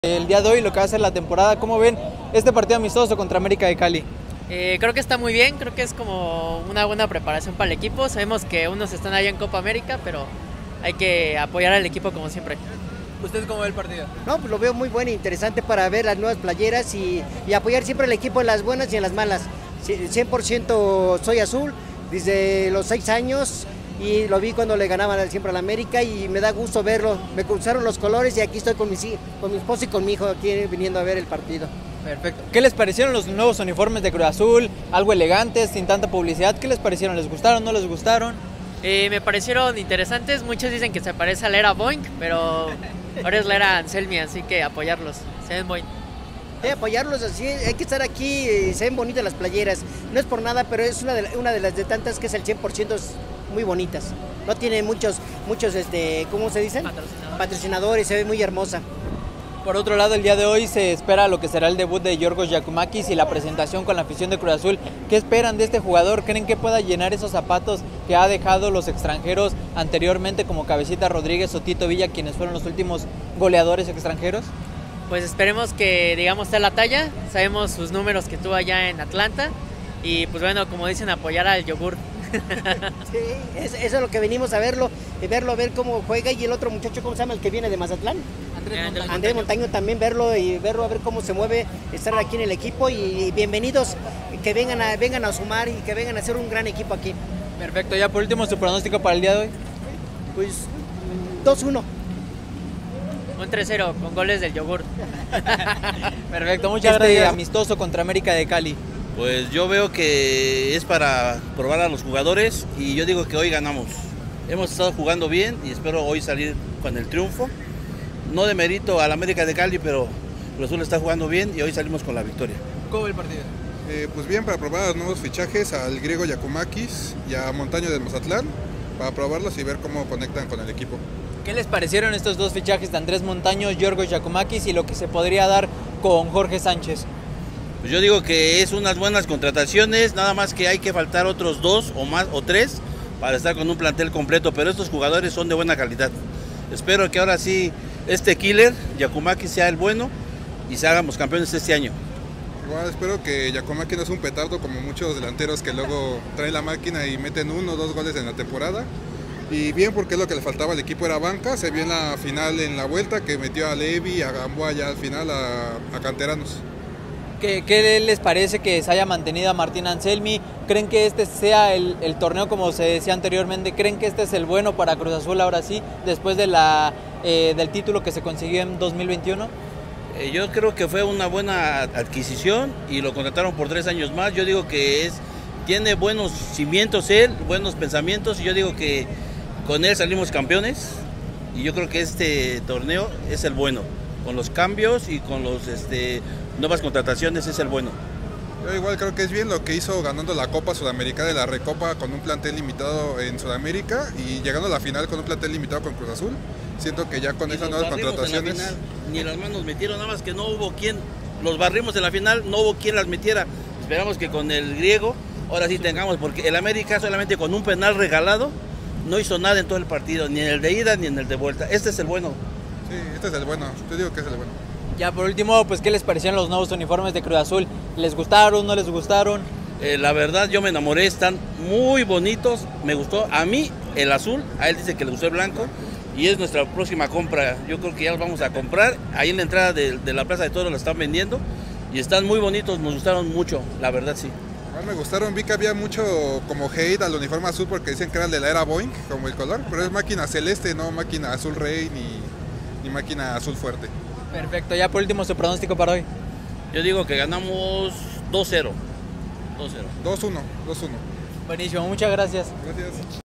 🚂 La OPINIÓN de los AFICIONADOS sobre los NUEVOS REFUERZOS y UNIFORMES de CRUZ AZUL 🔵🚂
🚂 Las entrevistas con los aficionados de Cruz Azul, previo al partido en contra del América de Cali 🔵🚂